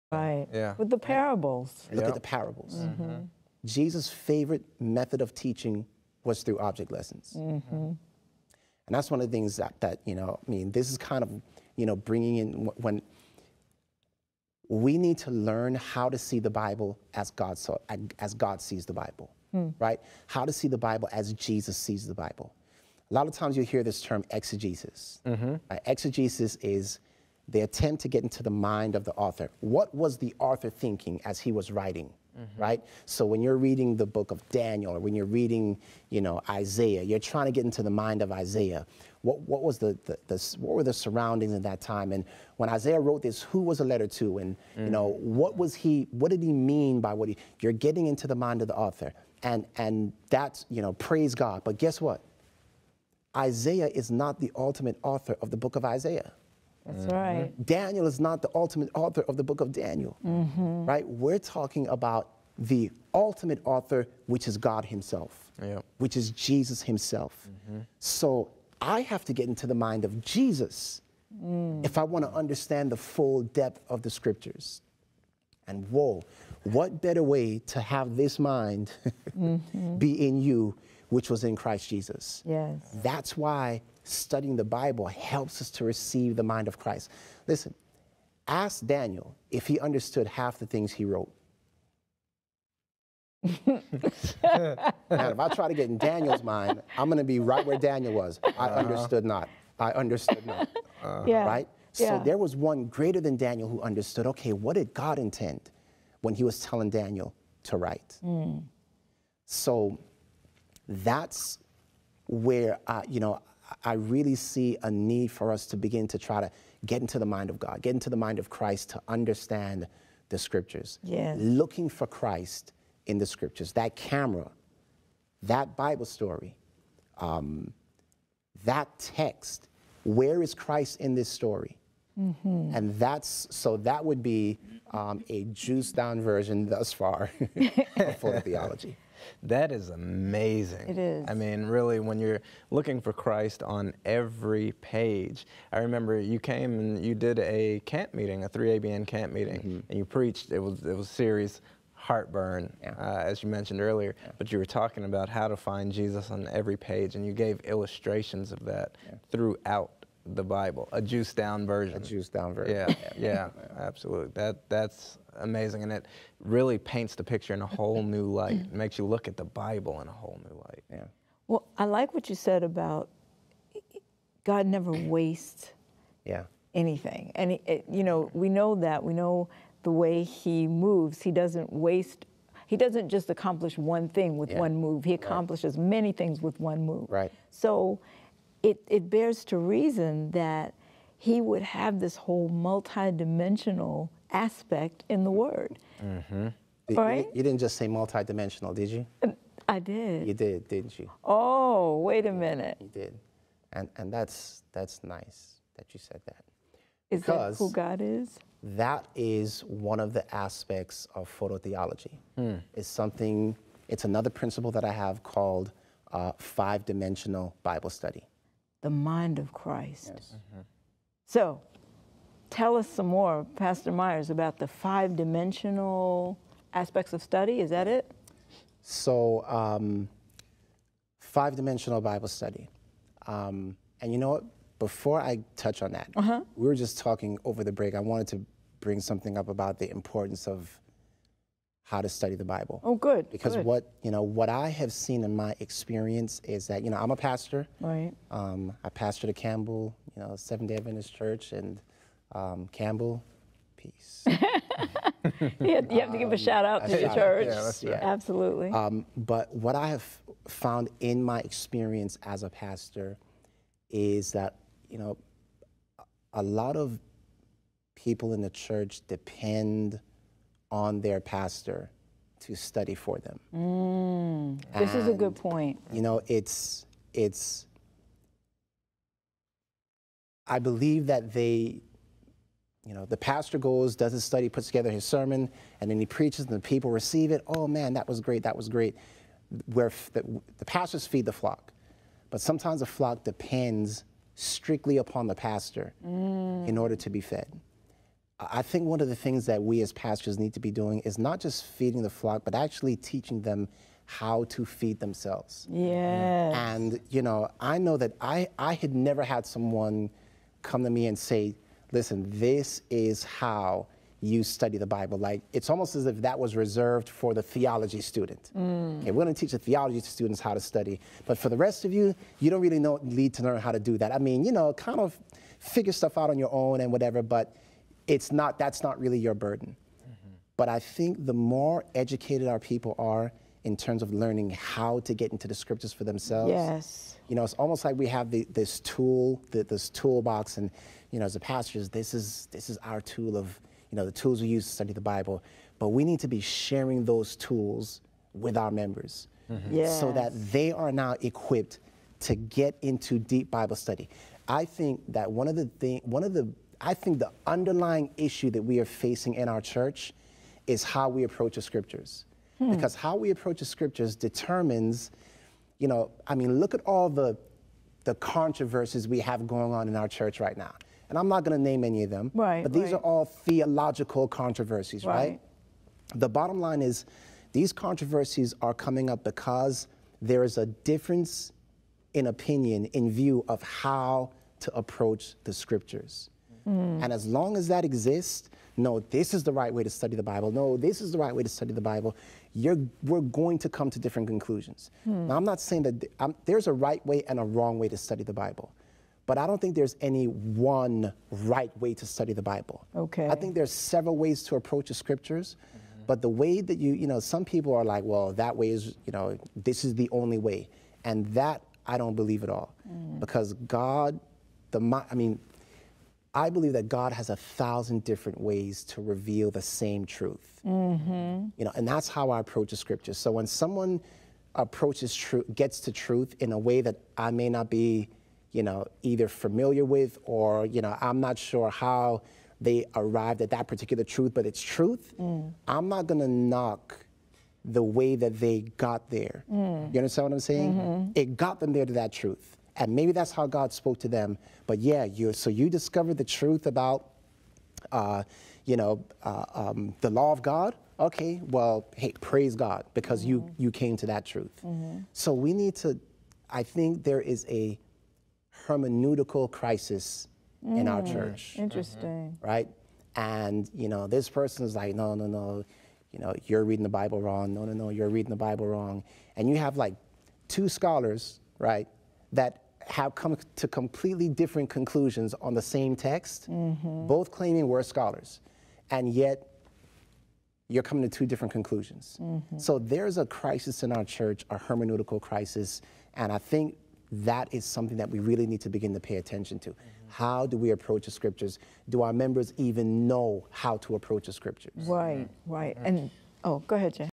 How to Study the Bible Jesus-esis Over Eisegesis | Pastor Ivor Myers
Christ should be the center and circumference of your Bible study.

That's partly how to study the Bible.
See a picture. Find Christ in it. Memorize it. Find a match.